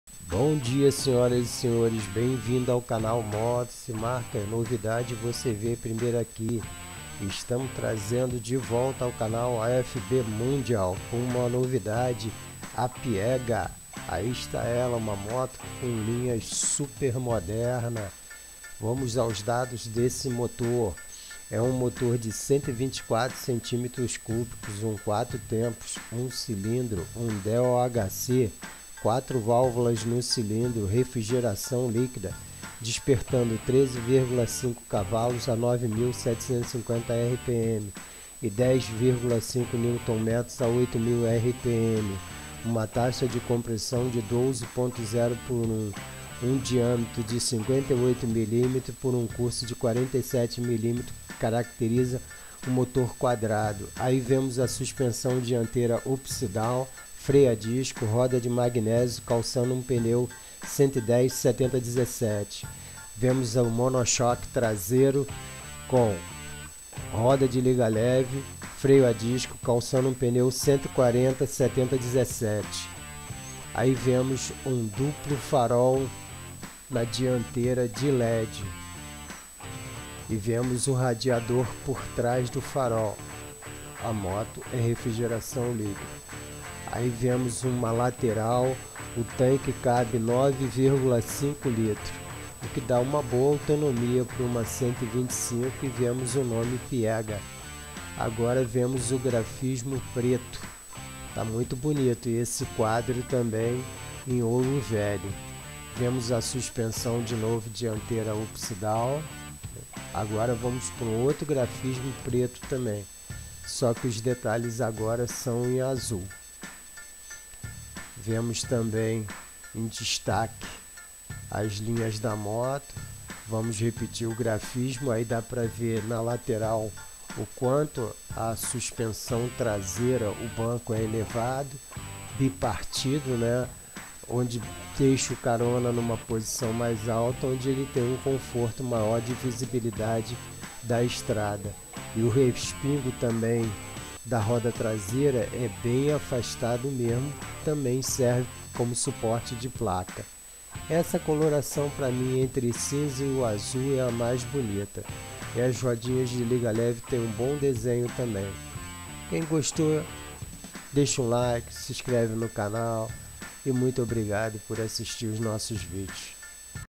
FB MONDIAL PIEGA 125 uma moto de linhas modernas, no Brasil você compraria? Comente
O vídeo mostra a Piega moto 125 CC da FB MONDIAL com motor monocilíndrico um DOHC com quatro válvulas e refrigeração líquida despertando 13,5 CV a 9750 rpm e 10,5 Nm a 8000 rpm, moto de linhas muito modernas